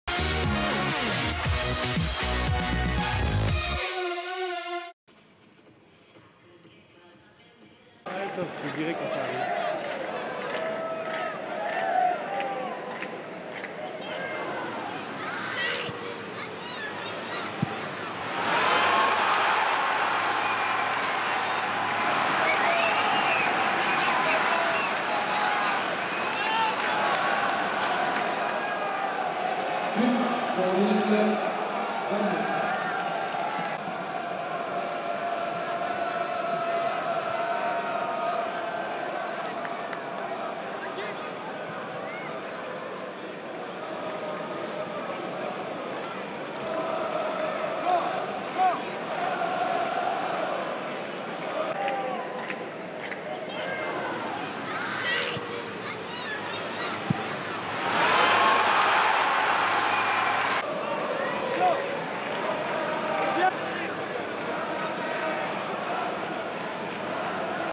But Florent BALMONT (40') / FC Metz - LOSC Lille (1-4) - (FCM - LOSC) / 2014-15
FC Metz - LOSC Lille (1-4) - But de Florent BALMONT (40'). Tous les buts de FC Metz - LOSC Lille en vidéo.

Stade Saint-Symphorien - samedi 23 mai 2015

Buteurs : Florent BALMONT (40' - LOSC Lille) / Idrissa GUEYE (41' - LOSC Lille) / Nolan ROUX (44' - LOSC Lille) / Nolan ROUX (69' pen - LOSC Lille) / José Luis PALOMINO (83' - FC Metz)